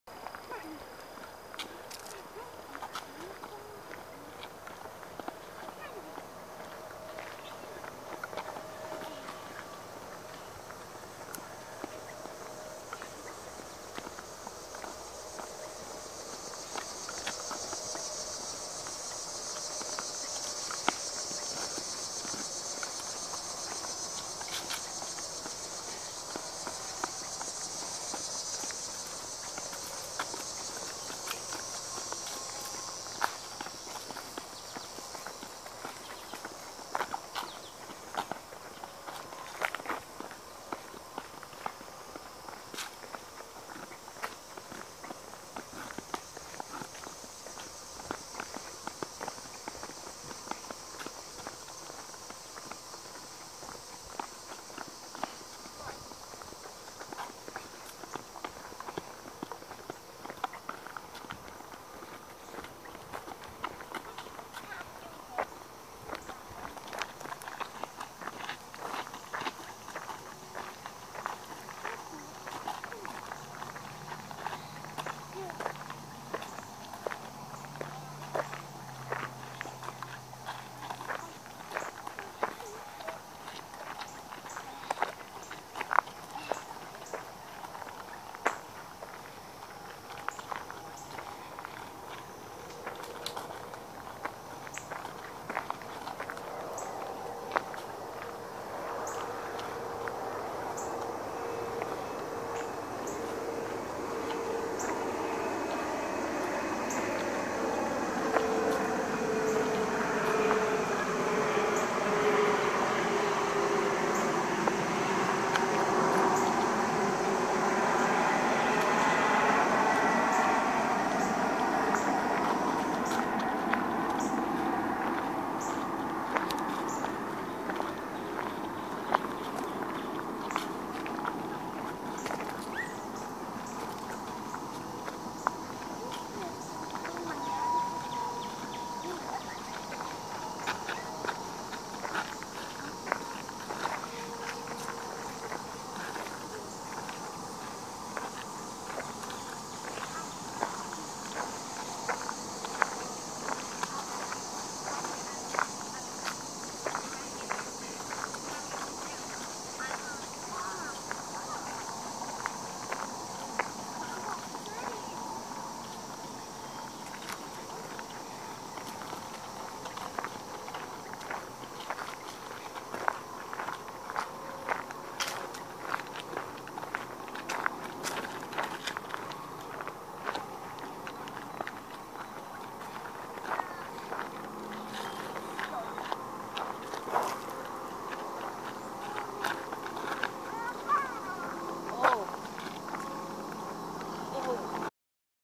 Into the Stinkiness
Twin sisters Nola Patrick and Logann Patrick traveling outdoors until we hit an area that had a strong plant odor that took your breath away. Filmed by Darrick Patrick on July 18th, 2018. Logann is wearing the grey shirt, Nola is in the blue shirt.